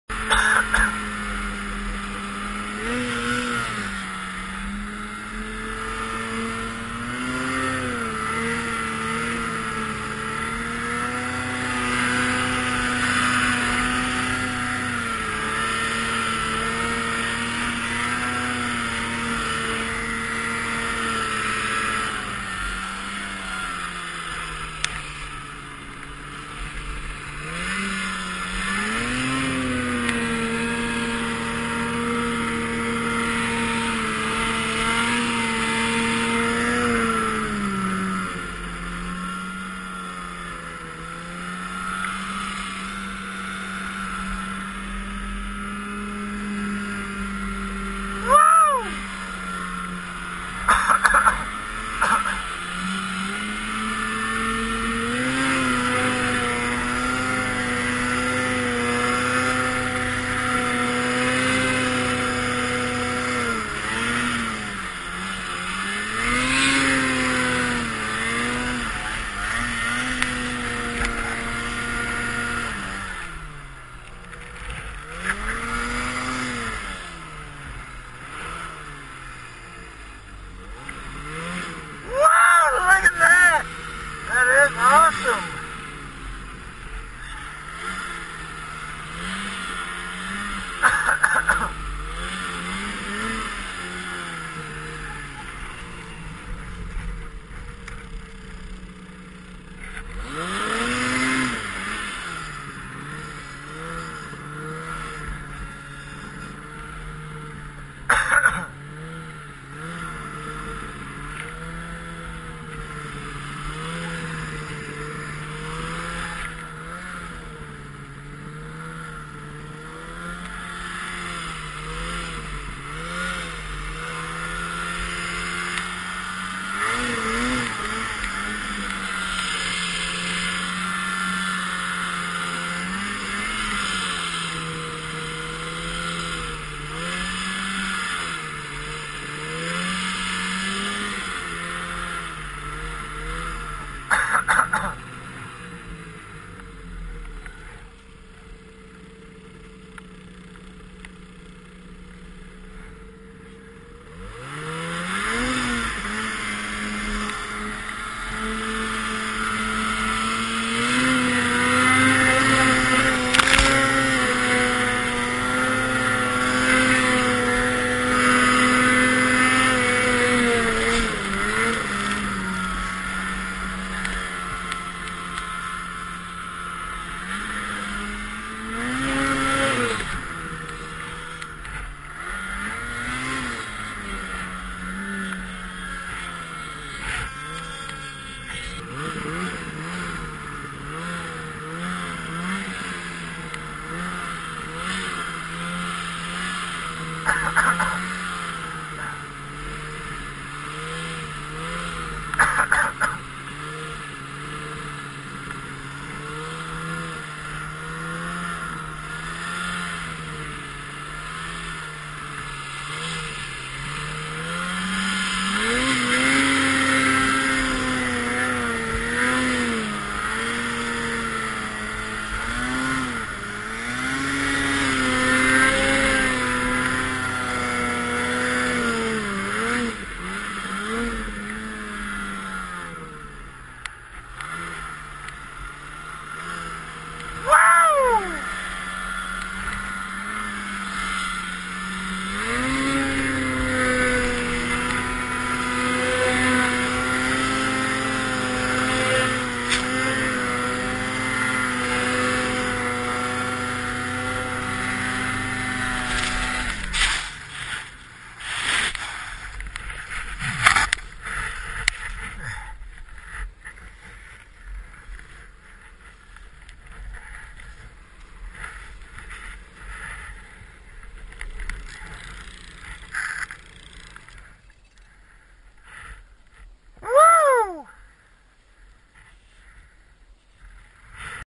Happy New Year
Playing in the bowl.  Allen in the trees on his 08 Ski doo summit xp 174.  Jan 1, 2011